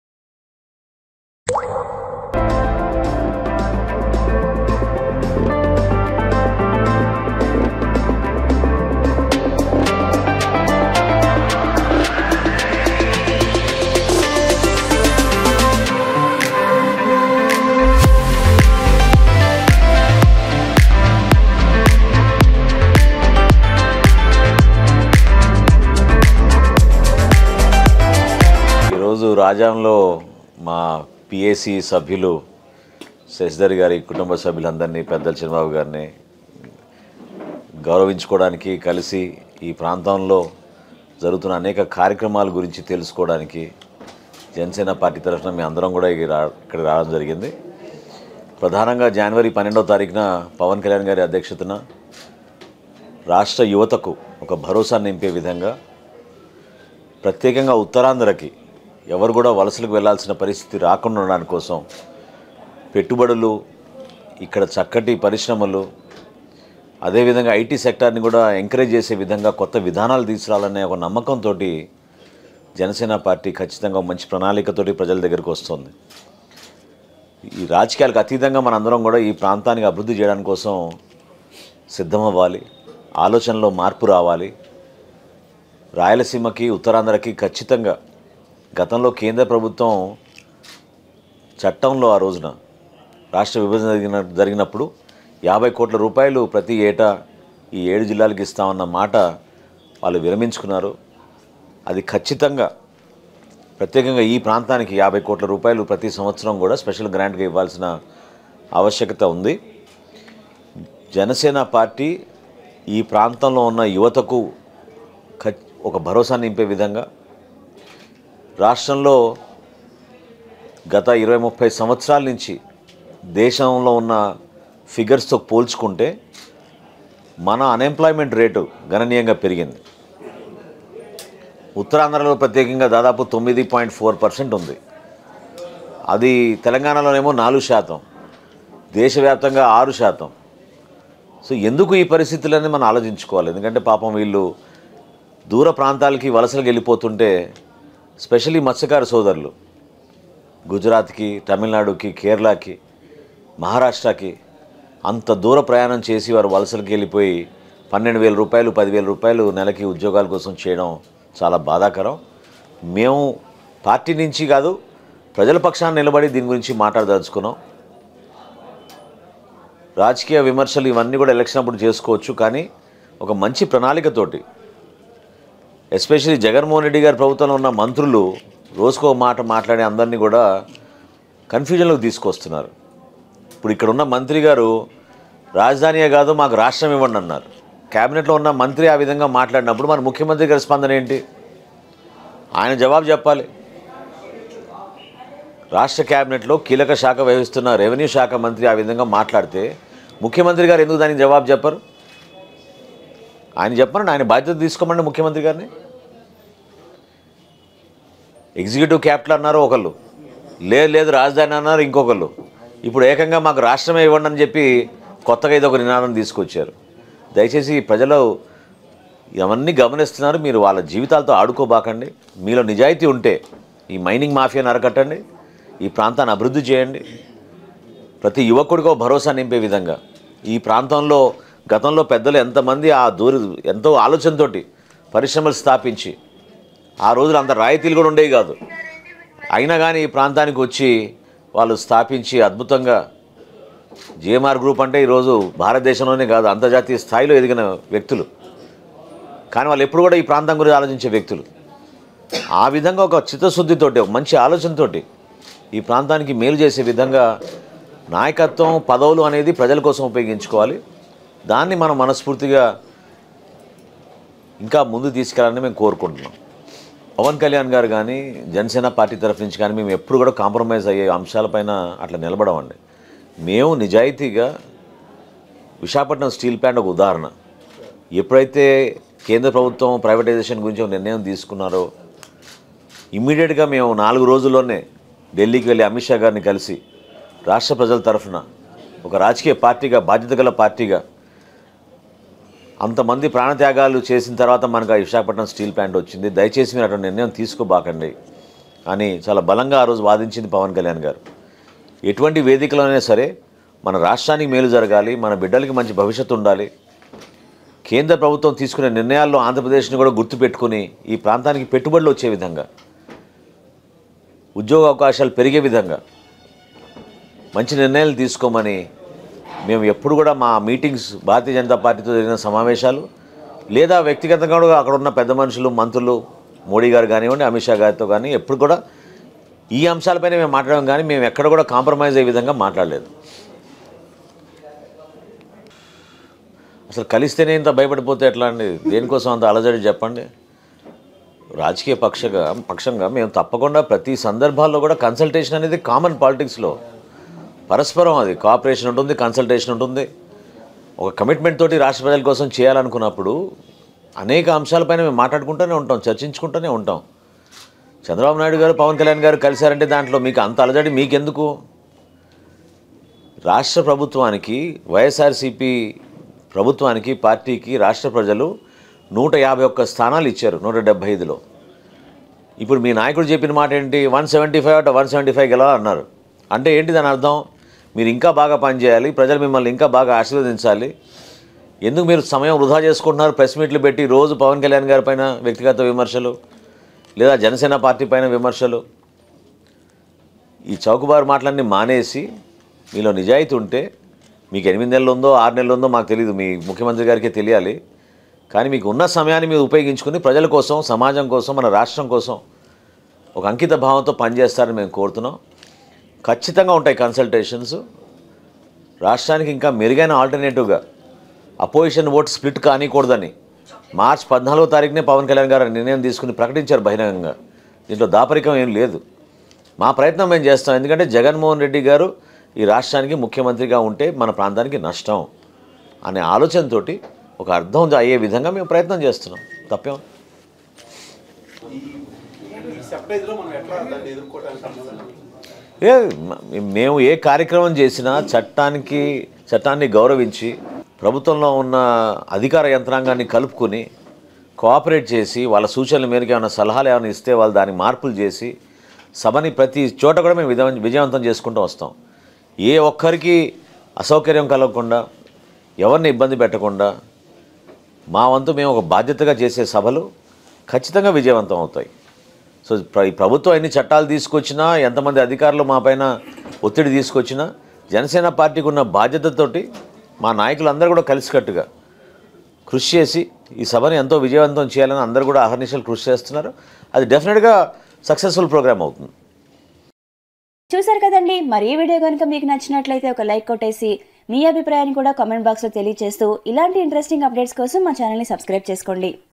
నాయకత్వం,పదవులు అనేవి ప్రజల కోసం ఉపయోగించాలి || Nadendla Manohar Press Meet || Ok Telugu
Watch Nadendla Manohar Talking With Media Video In Ok Telugu